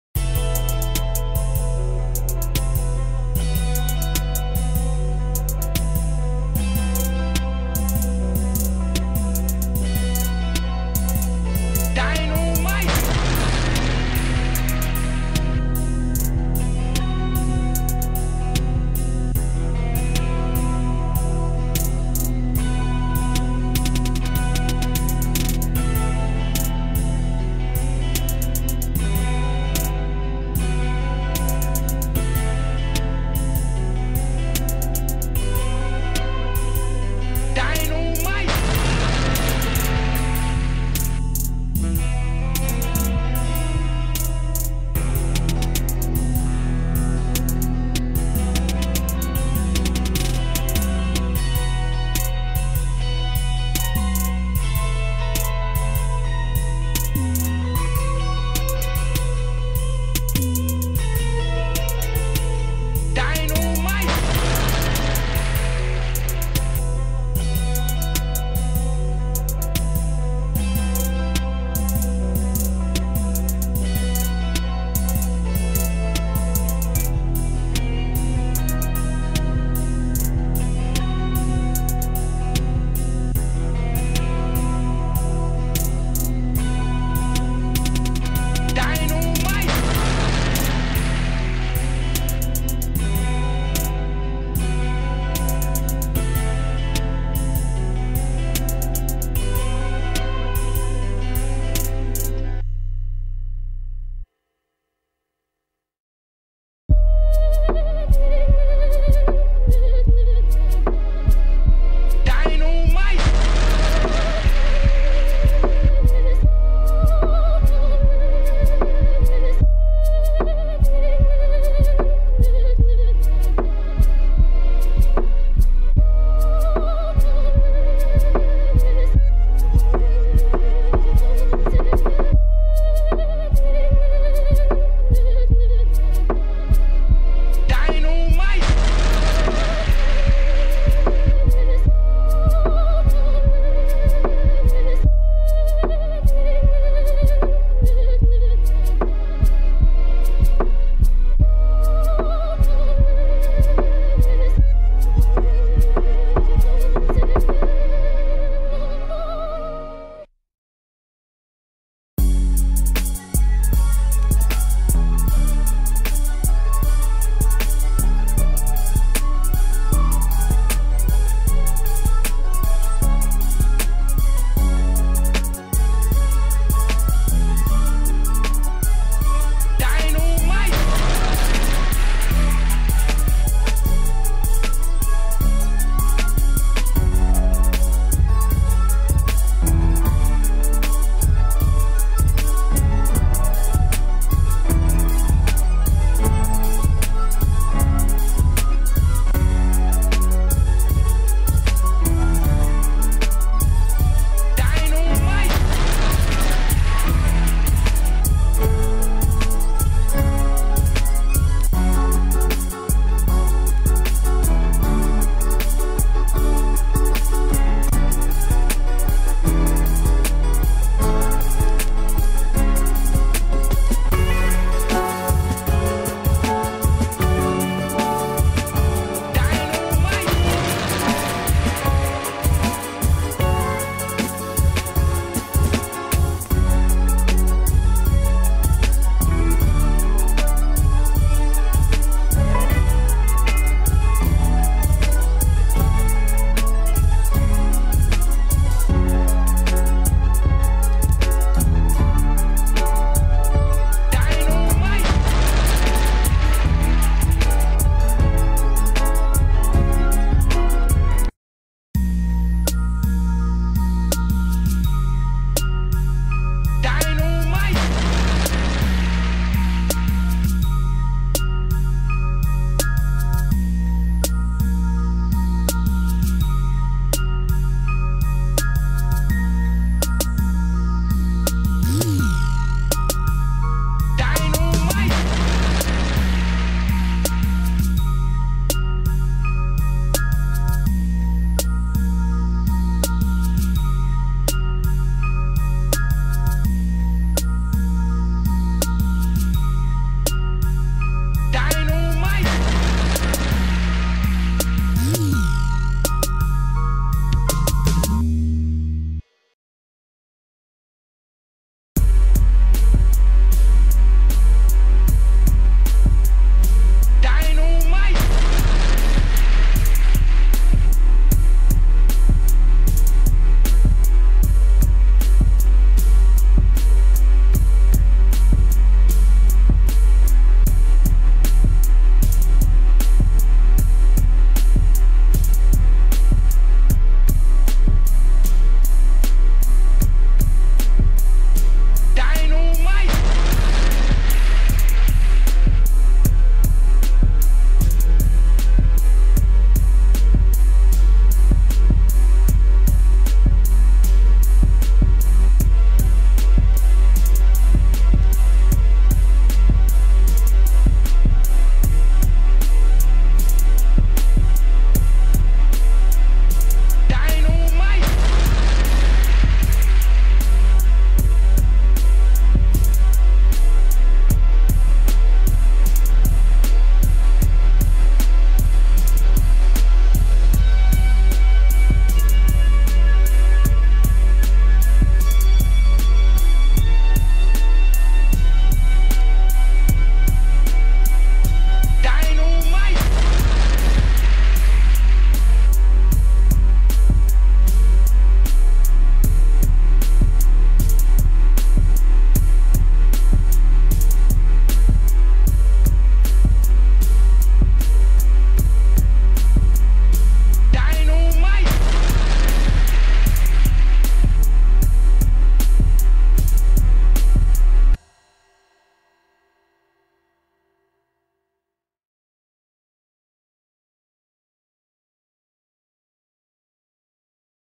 NFL TOP FIVE (Buffalo Bills) Rap Beats
NFL TOP FIVE (Buffalo Bills) Rap Beats.Beats By Ry-Fly.Greatest Buffalo Bills Players Of All Tyme